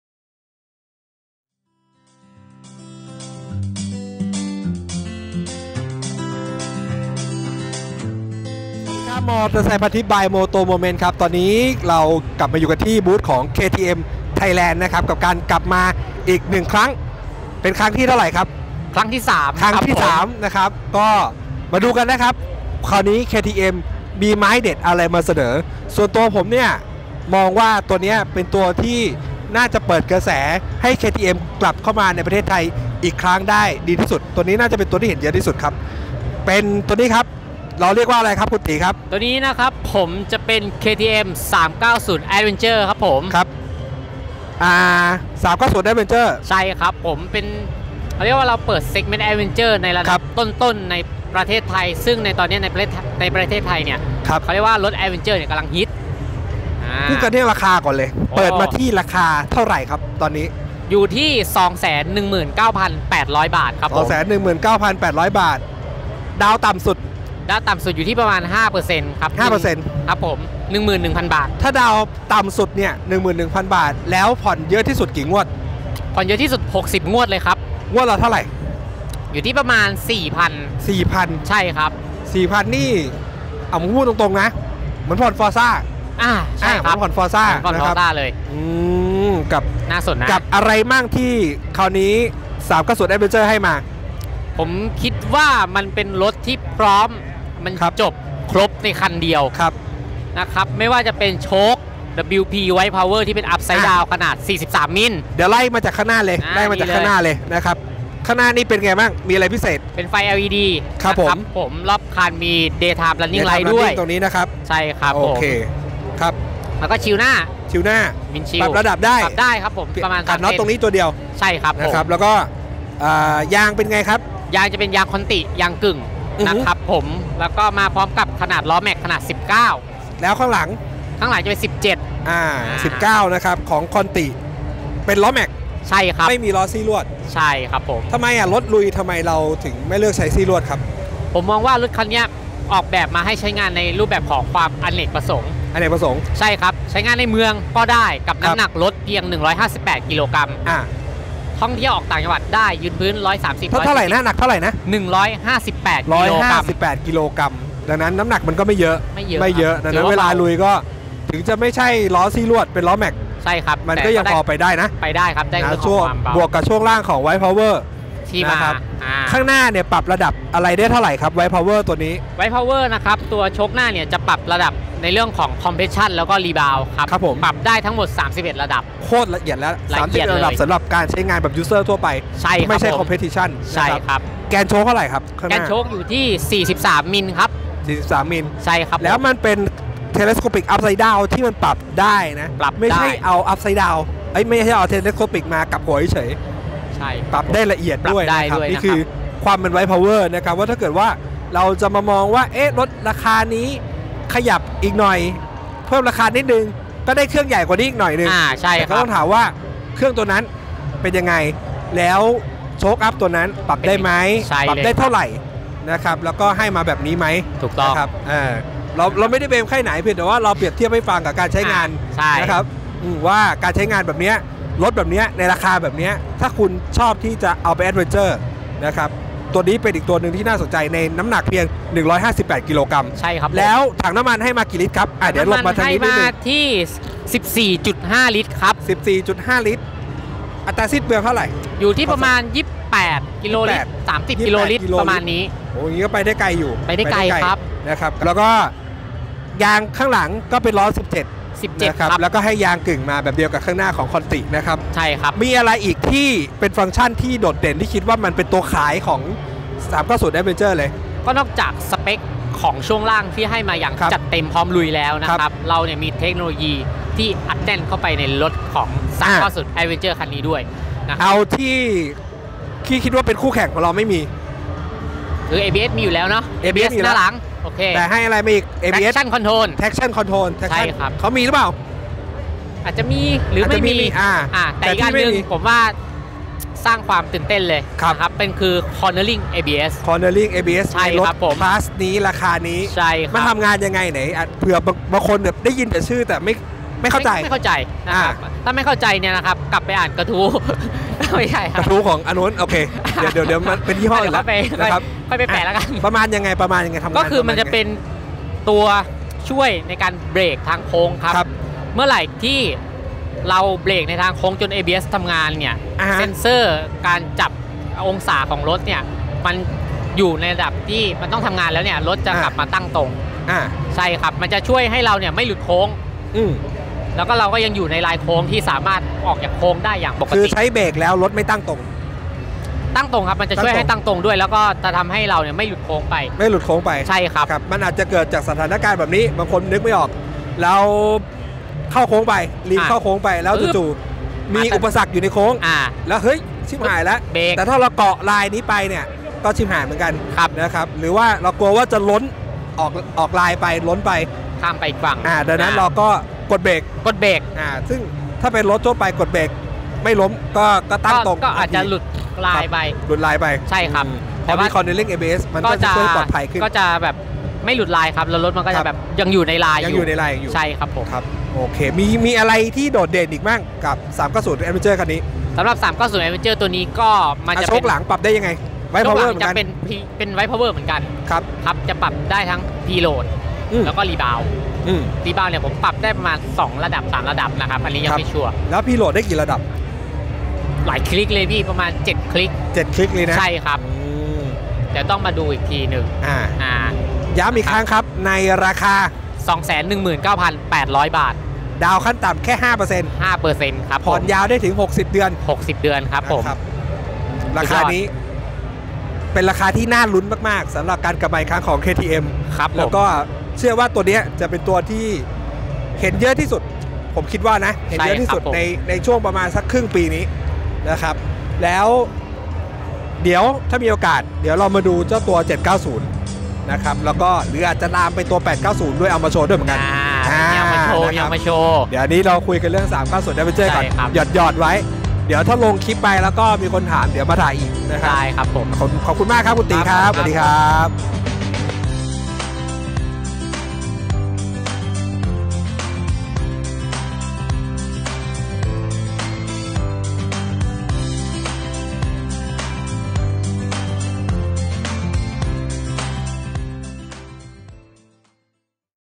KTM 390 Adventure ราคาเท่าไร มีอะไรดี ทำไมต้องซื้อ
คุยกับเซลล์ตี๋ แห่ง KTM Thailand ถามกันตรงๆไม่มีอ้อม KTM 390 Adventure ราคาเท่าไร มีอะไรดี ทำไมต้องซื้อ ดาวน์ต่ำสุดเท่าไร ผ่อนได้นานสุดกี่เดือน ฟีเจอร์เด็ดๆที่เป็นจุดขาย ฟันธง!!